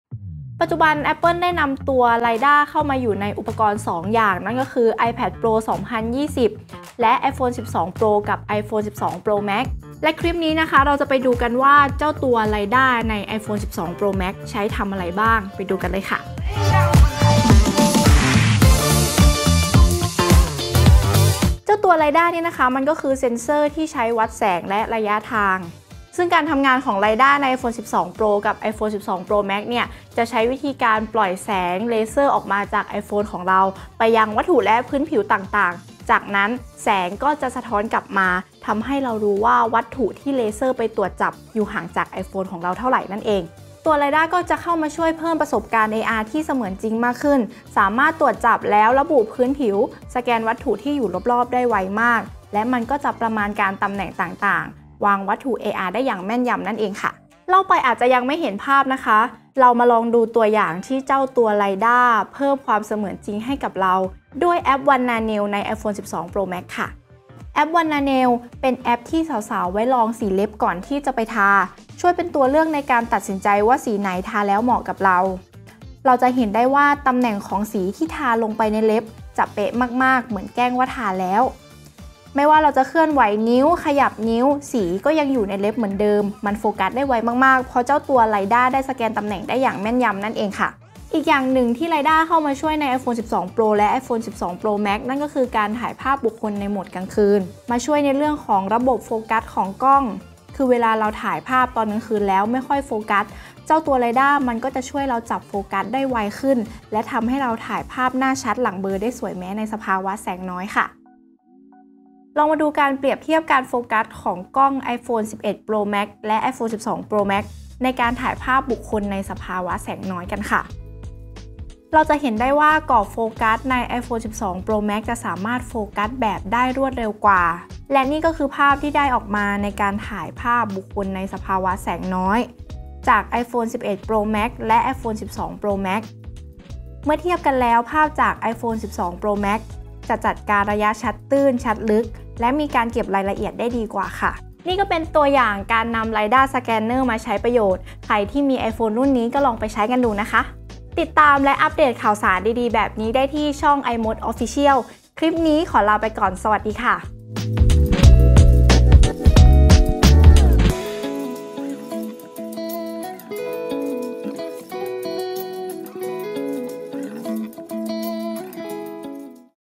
[iMoD] LiDAR Scanner ใน iPhone 12 Pro, iPhone 12 Pro Max คืออะไร ใช้ทำอะไรได้บ้าง
iPhone 12 Pro, iPhone 12 Pro Max เป็น iPhone ซีรีส์แรกที่มี LiDAR Scanner ที่กล้องหลัง, เรามาดูกันว่า
LiDAR Scanner คืออะไร และใช้ทำอะไรได้บ้าง

📌 หัวข้อ

00:00 - Intro
00:20 - LiDAR Scanner คืออะไร
00:44 - ประโยชน์ของ LiDAR Scanner
00:57 - ตัวอย่างการใช้งานด้วยแอป WANNA NAILS
01:31 - ช่วยเรื่องถ่ายภาพบุคคลในที่แสงน้อย, โหมดกลางคืน
01:52 - เปรียบเทียบความเร็วการโฟกัสระหว่าง iPhone 11 Pro Max และ iPhone 12 Pro Max